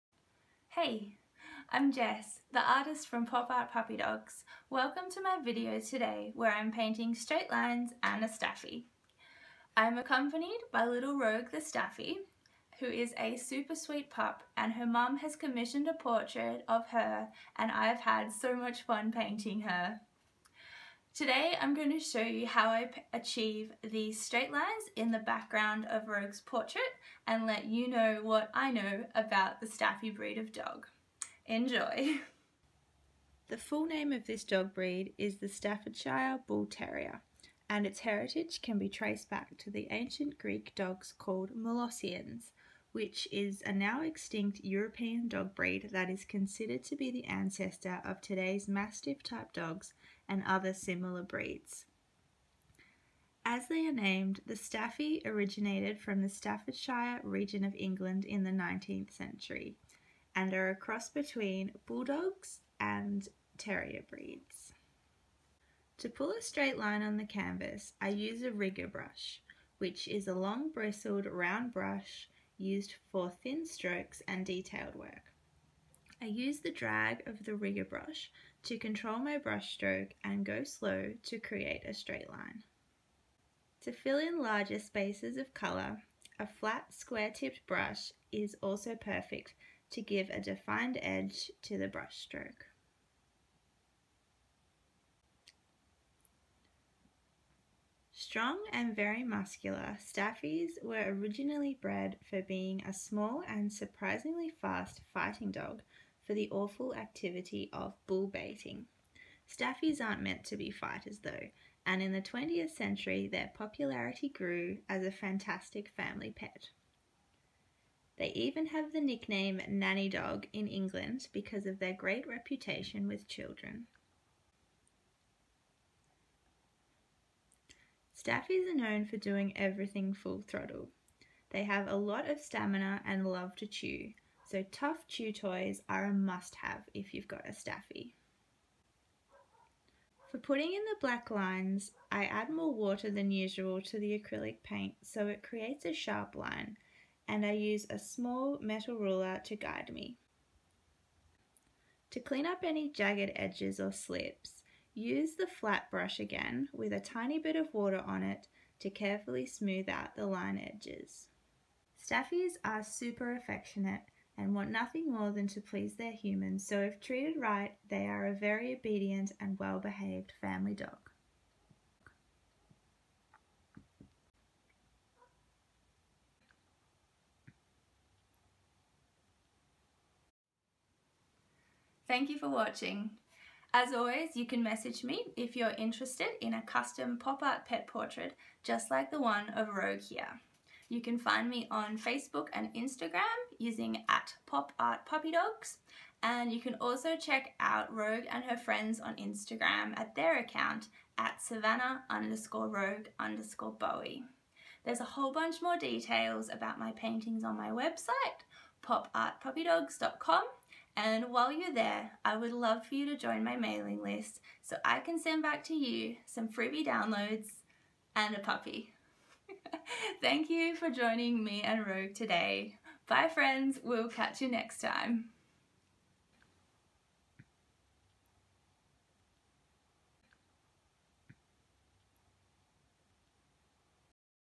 Painting Straight Lines and a Staffordshire Bull Terrier
I'll show you how I paint the straight lines in my pop art pet portraits, featuring, the one and only, Rogue the Staffy. Listen in for some fun facts about the staffy breed of dog too.

If you would love to see more of my art, please feel welcome to join my mailing list for some freebie downloads and updates on my world of pets and art.